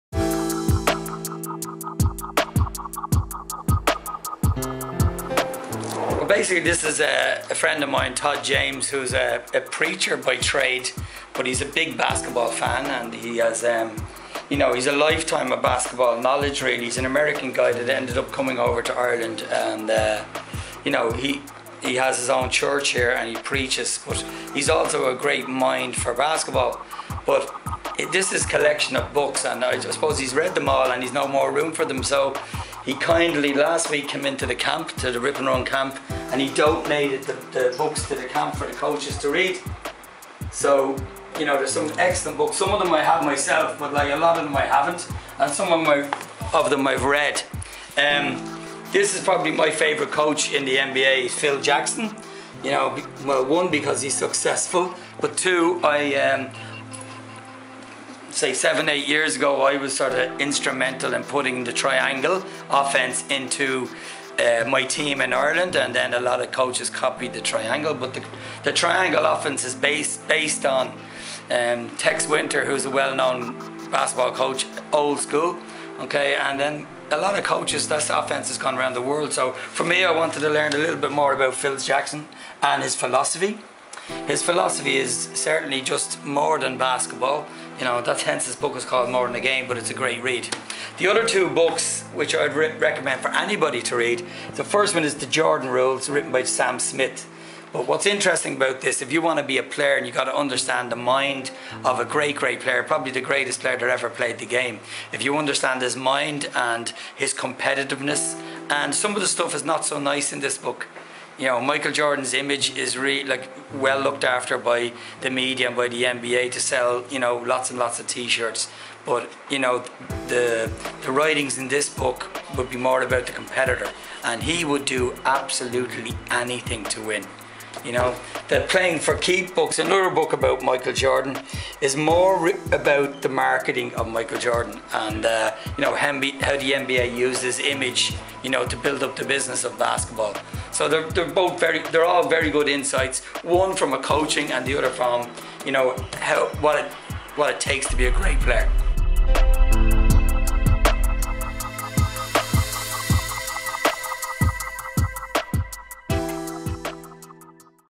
Mark Ingle - Reading Basketball
Todd James Basketball Coach shares his best basketball books with the RipnRun coaching staff at RipnRun 2011.Mark Ingle talks about the books.